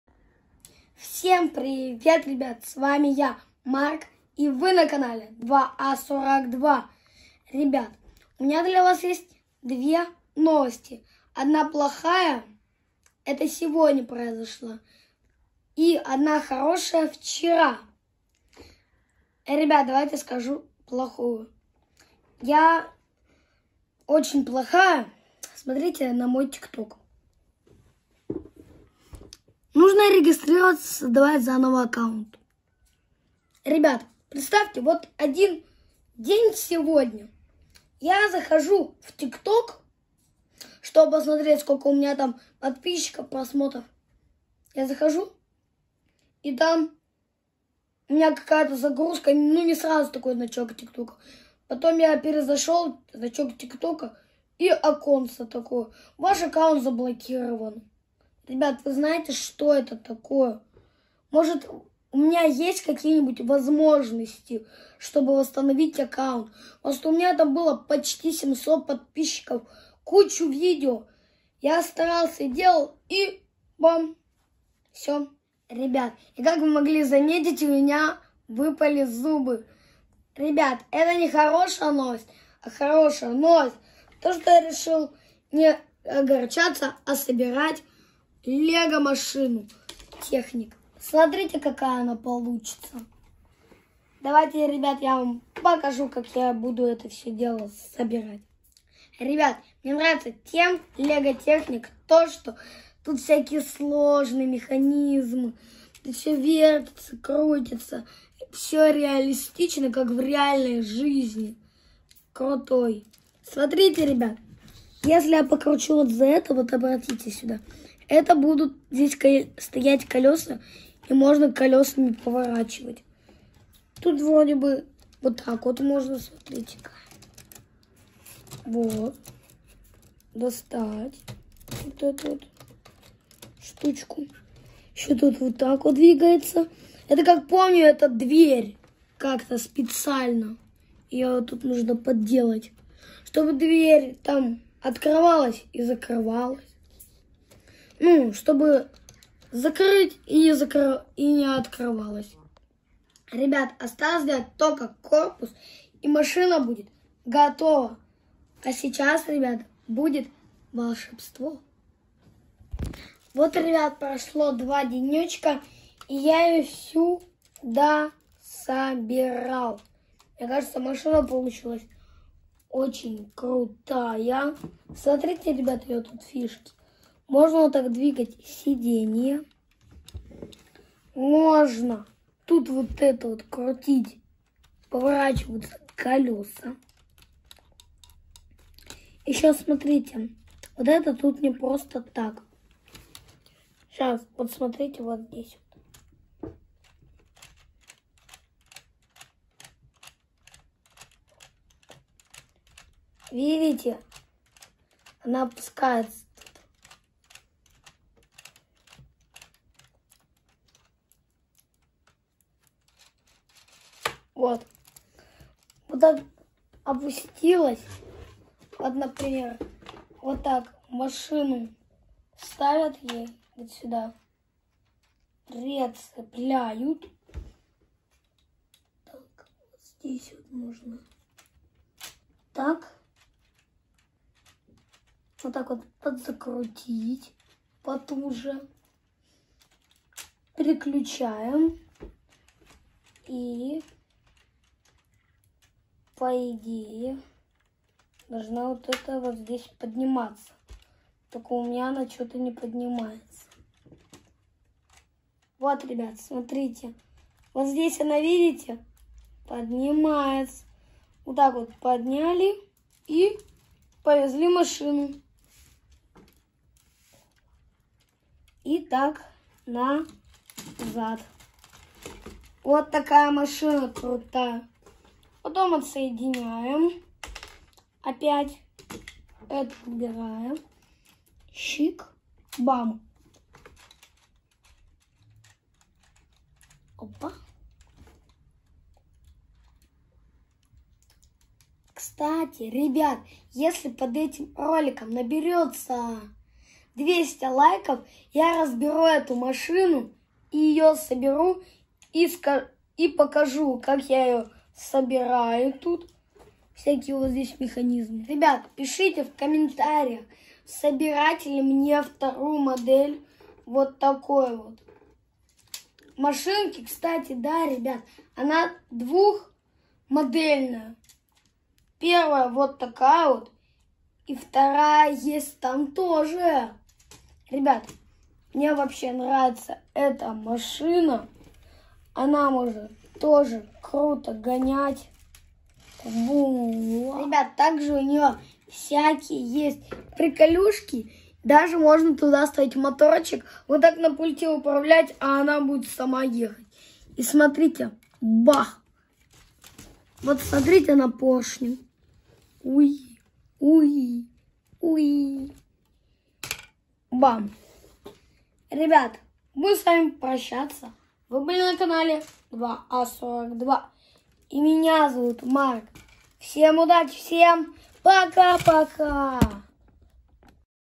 Лего техник 9395 / собираем крутую машину из серии Лего
Lego technic 9395 что же интересного в этой модели / обзор машины